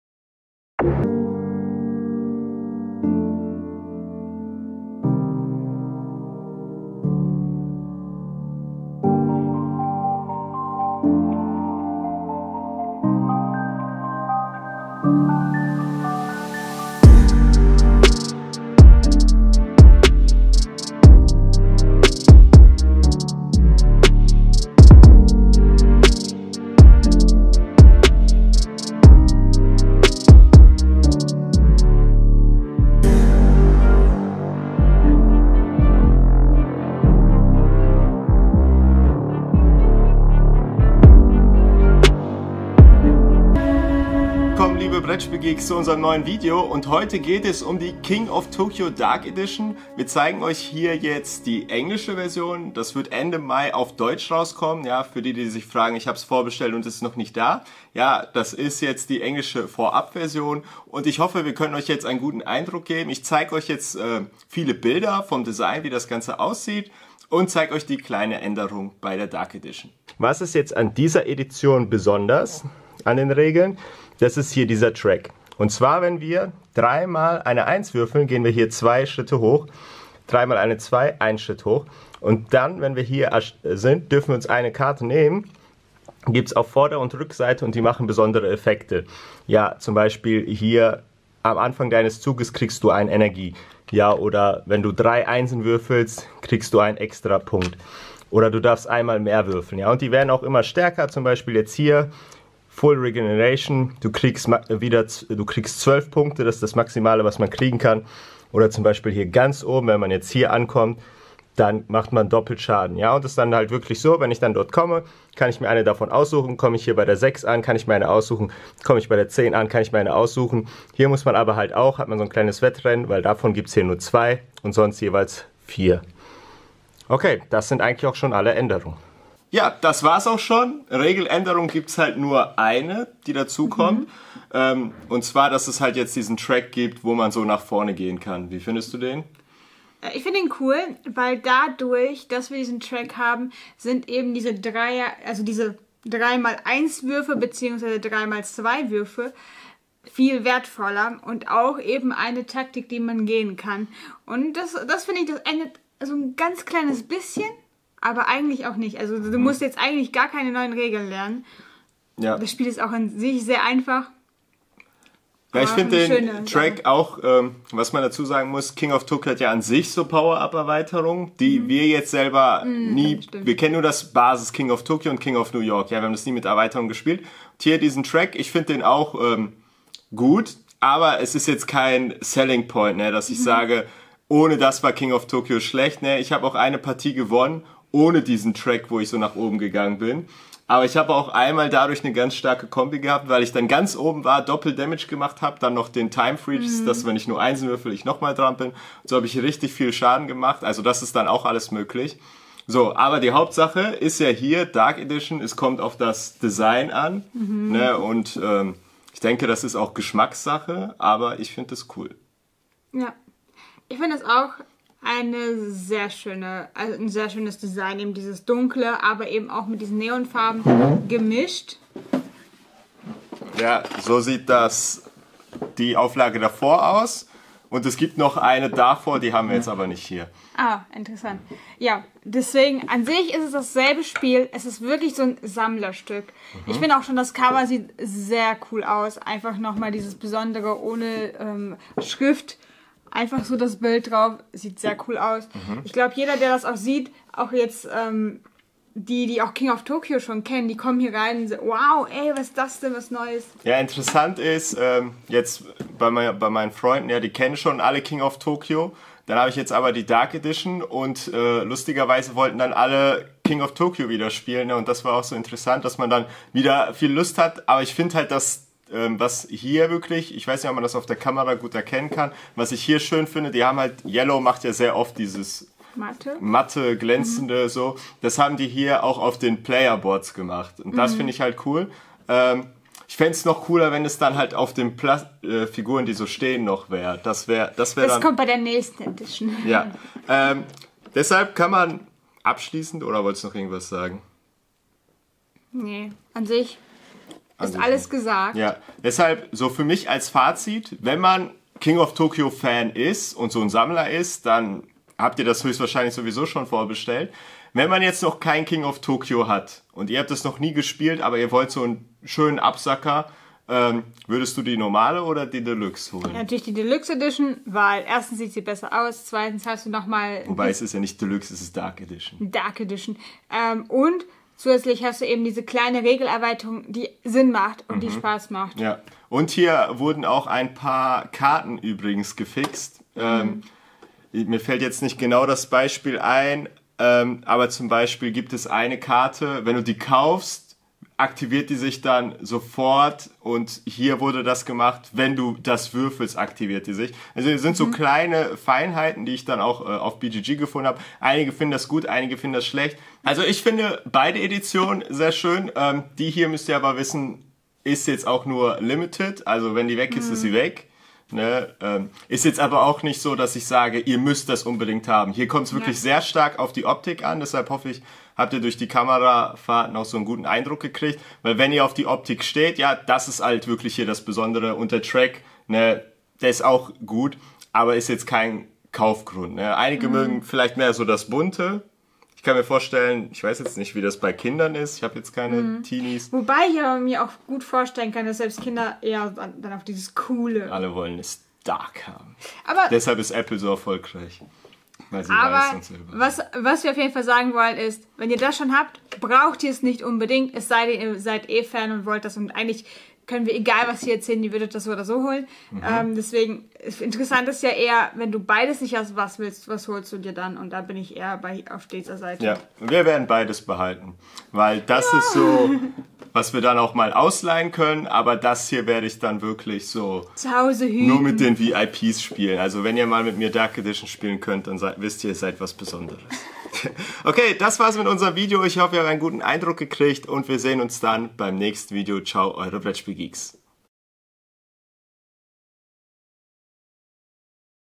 King of Tokyo Dark Edition - Lohnt es sich? - Brettspiele 2020
Heute schauen wir uns die neue Dark Edition von King of Tokyo an.
Viel Spaß :)

Für dieses Video stand uns ein Rezensionsexemplar zur verfügung.